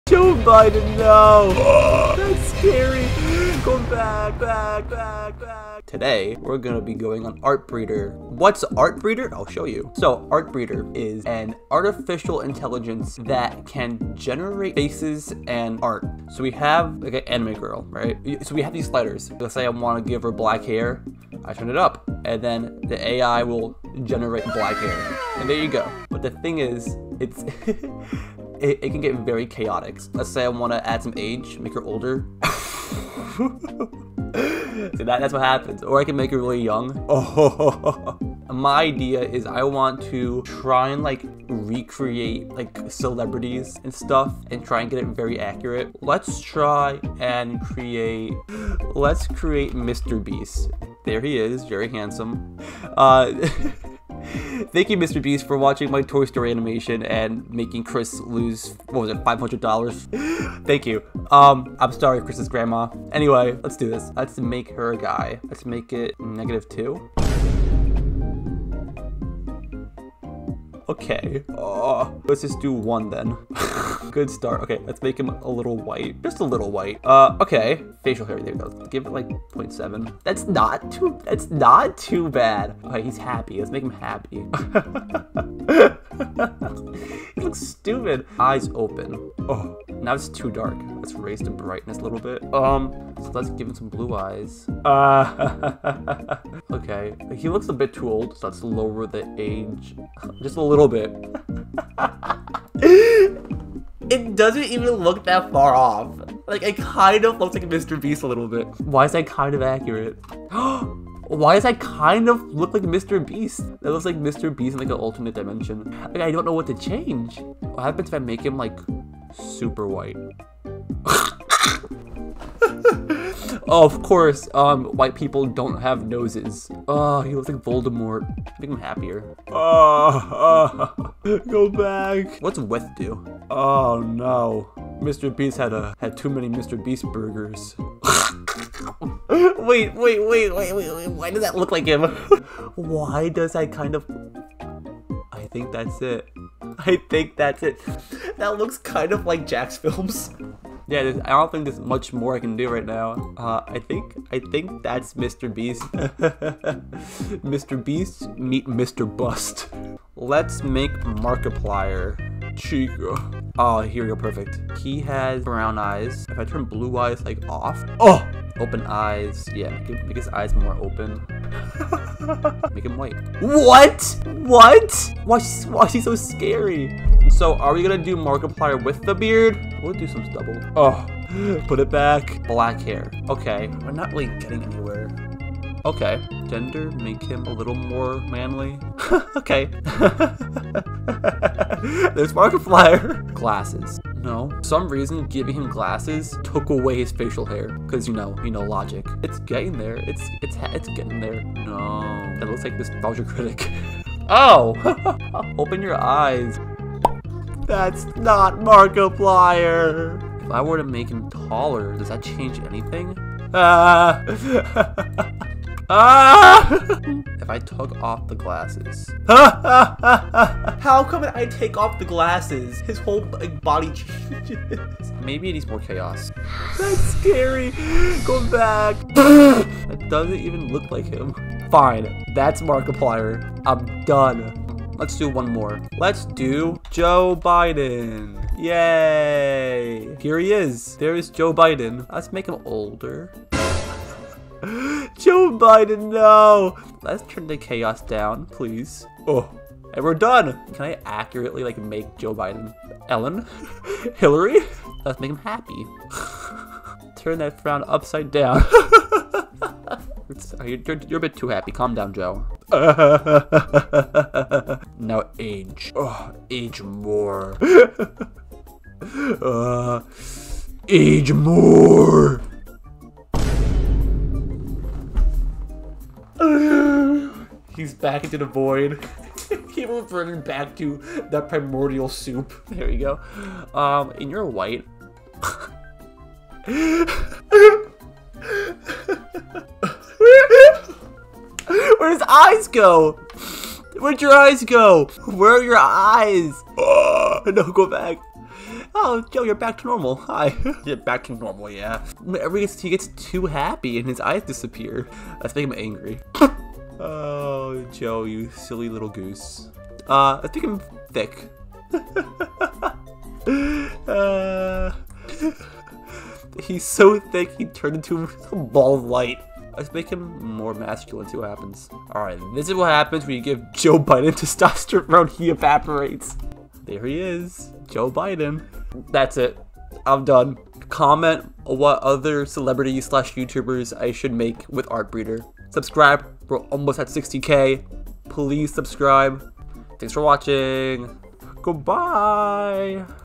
AI RECREATES YOUTUBERS (ARTBREEDER)
Mr. Beast if you're watching please let me animate something for you.

I know these aren't accurate. Artbreeder currently has many limitations but it is still a very interesting website to mess around with.